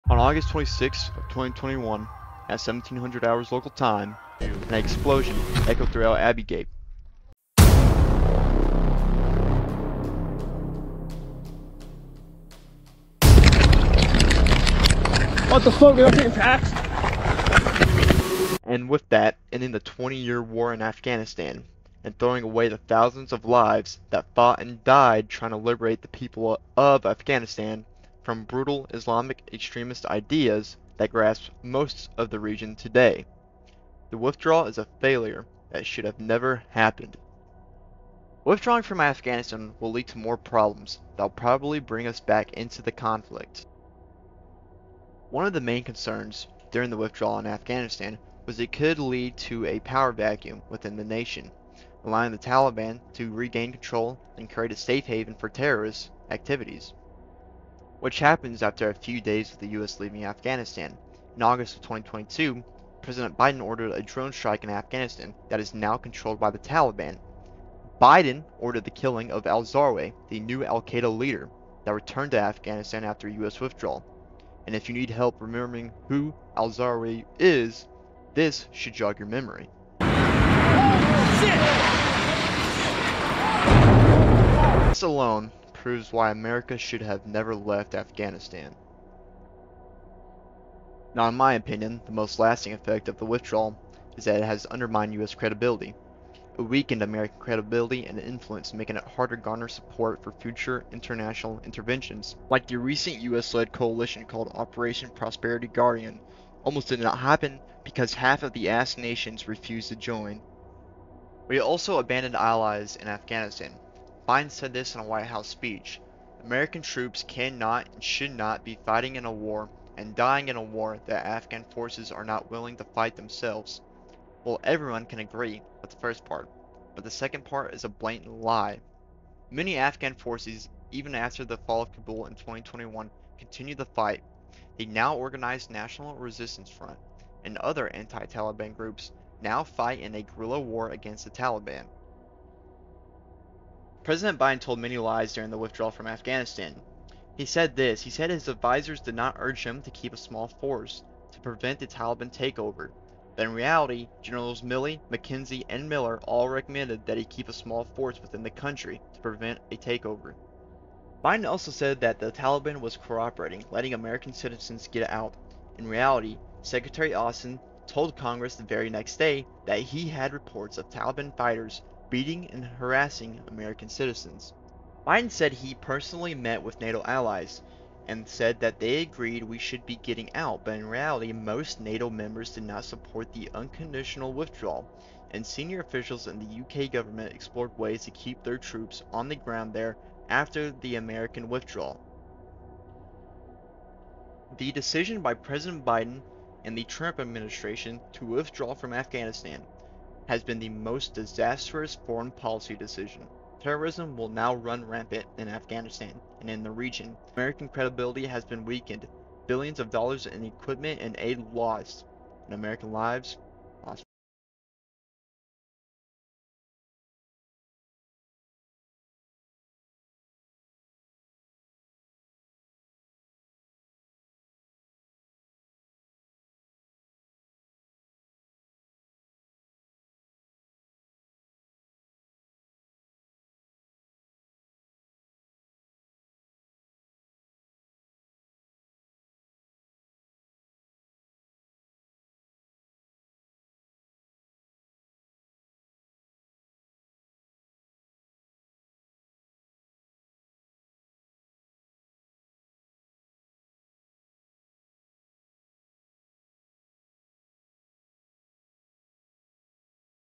America should have never left Afghanistan
On 26 August 2021, at 17:50 local time. An explosion echoed throughout Abbey Gate killing 13 U.S. service members. And with that ending the twenty-year war in Afghanistan, and throwing away the thousands of lives that fought and died trying to liberate the people of Afghanistan from brutal Islamic extremist ideas that grasp most of the region today. The withdrawal is a failure that should have never happened.